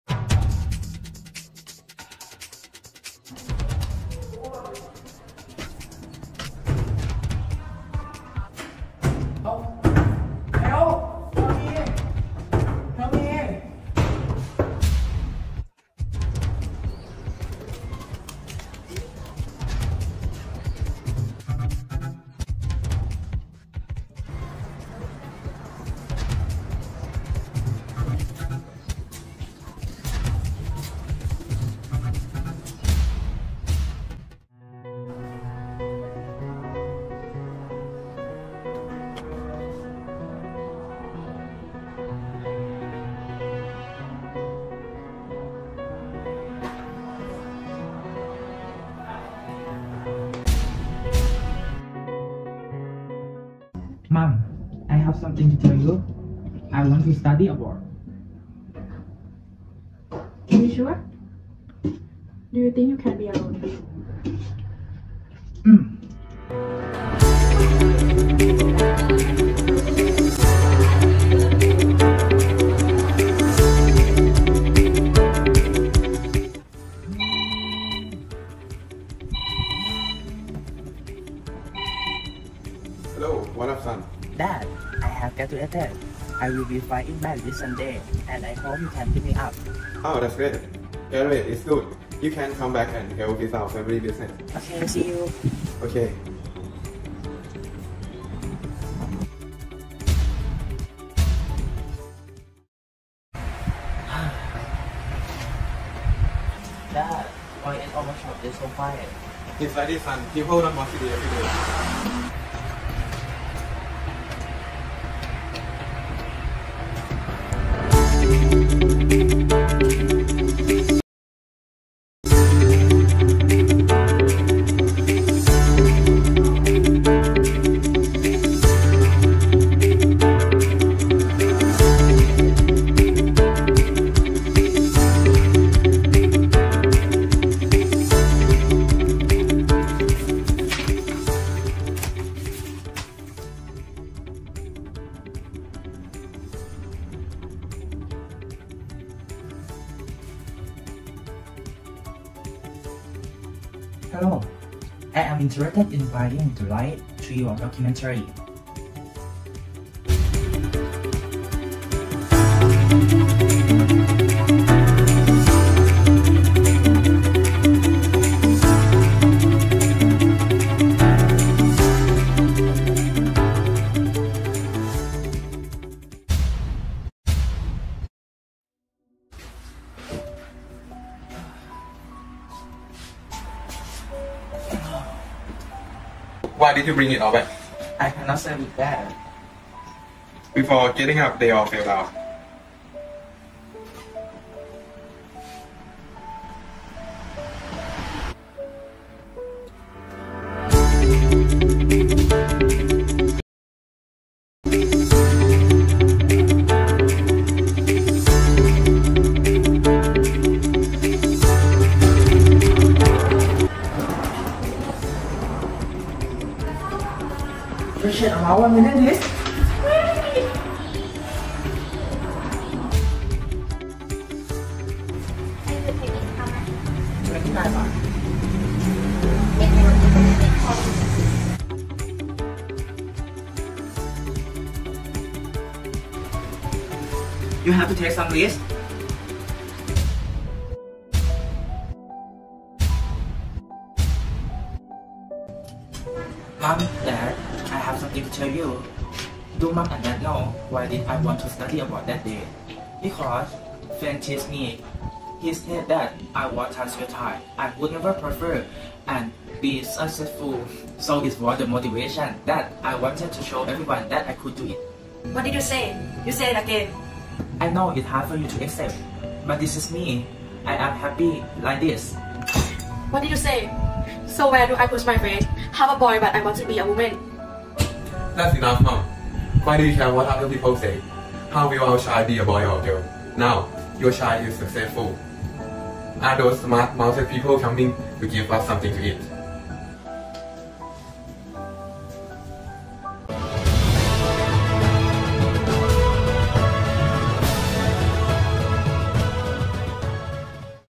Biography of Anne Jakrajutatip by BRR 5/8
Here's an output from my students:

Group 8
Members:
Patnaree Boonsakchalerm M.5/8 No.13A
Sasitorn Nimcharoen M.5/8 No.16A
Phattharaphon Wachirasakul M.5/8 No.3B
Pacharapa Kaewjinda M.5/8 No.11B
Ruethaiphat Chitchamnong M.5/8 No.14B